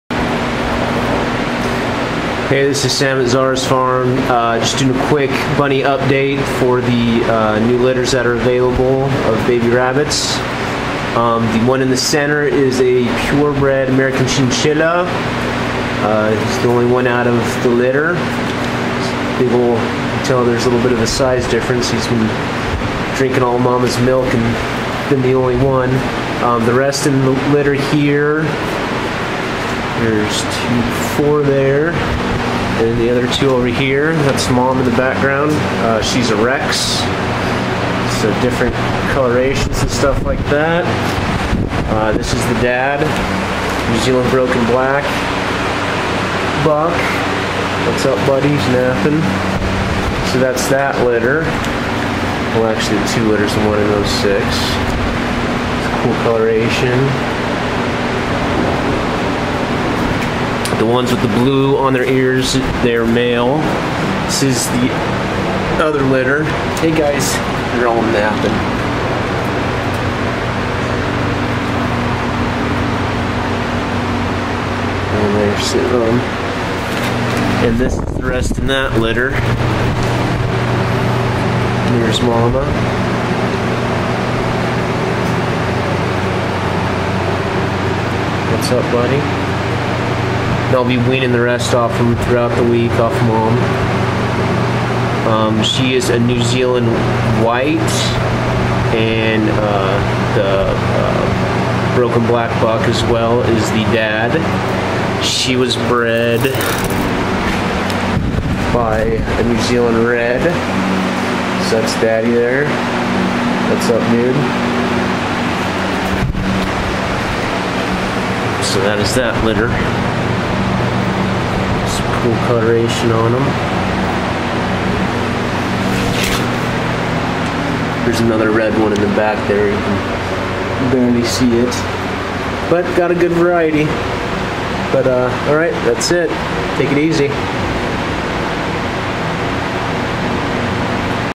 Bunny Update 9/26/2017
3 litters of New Zealand mixed rabbits. I have a great variety and if you are looking for an unrelated pair or trio I can do that as well.